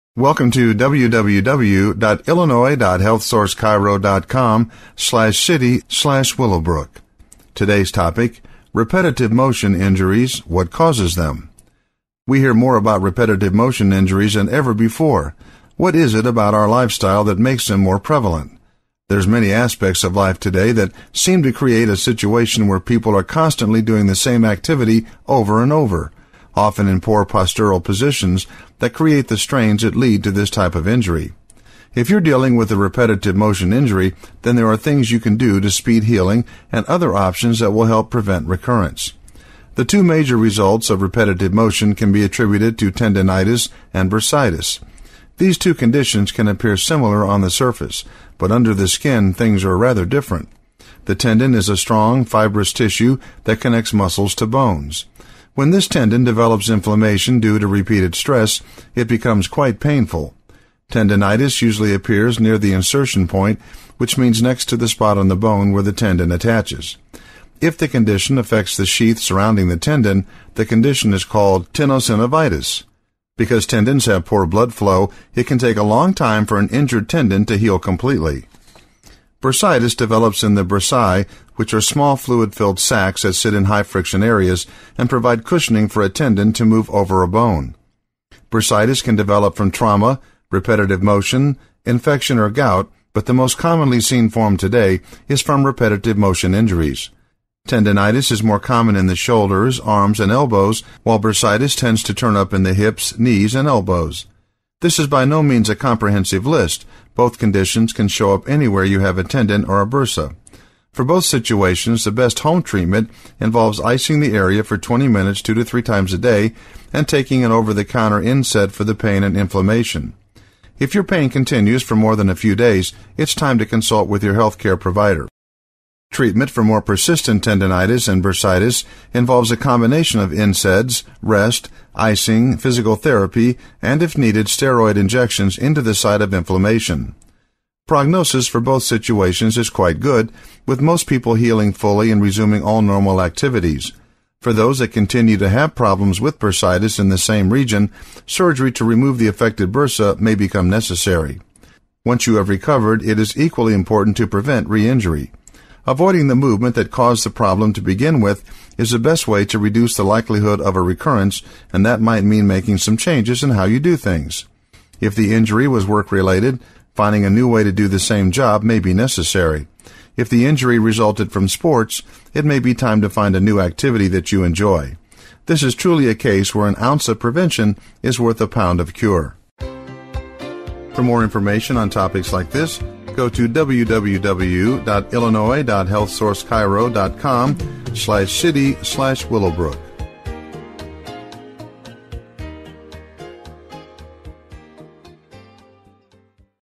Chiropractor in Willowbrook IL | Repetitive Motion Injuries
and Chris Mersinger Present Neck Pain And Back Pain in Willowbrook IL, Today's Topic, "Repetitive Motion Injuries; What Causes Them " Go